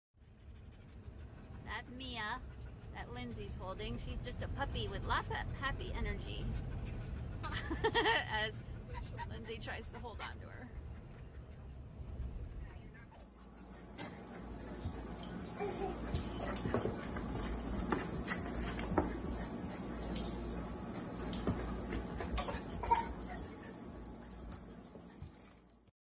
Mia July 2020 w Passion 4 Paws Vermont
IN VERMONT
Meet Mia (of Tulip, Buster, Petunia, Rory, and Mia)!
Mia is (tbd-we think under a year) old (approx. DOB tbd), female, 30 lbs., vetted, is or will be spayed, and micro-chipped. Mia is just a young puppy with lots of energy and happiness! She will need to learn the basics but is VERY smart! She seems to get along with other dogs; she came from a neglectful home.
If you are interested in adopting Mia please fill out an Application (please make sure you have the Vermont site not CT) - thanks!
P4P-072020?
Please note Fosters have 1st option of Adoption